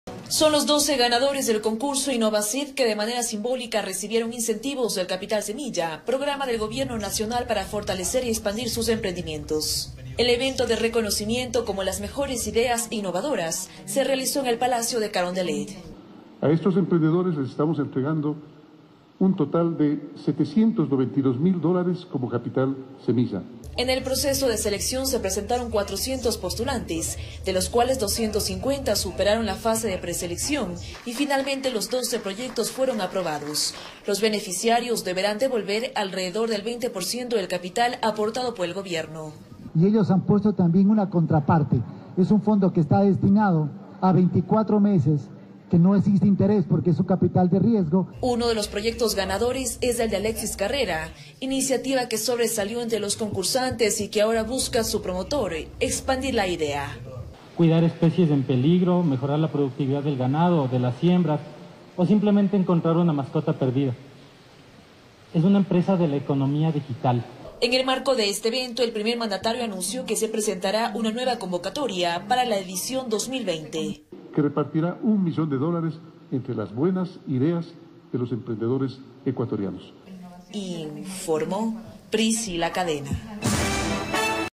12 ganadores del concurso INNOVACyT recibieron incentivo
Las autoridades recordaron que INNOVACyT forma parte del Fideicomiso Fondo Capital de Riesgo.

El presidente de la República, Lenín Moreno, entregó cerca de USD 792.000 de capital semilla a los 12 ganadores de INNOVAC 2019 e hizo la convocatoria a la edición 2020, proceso en el que se otorgará un millón de dólares.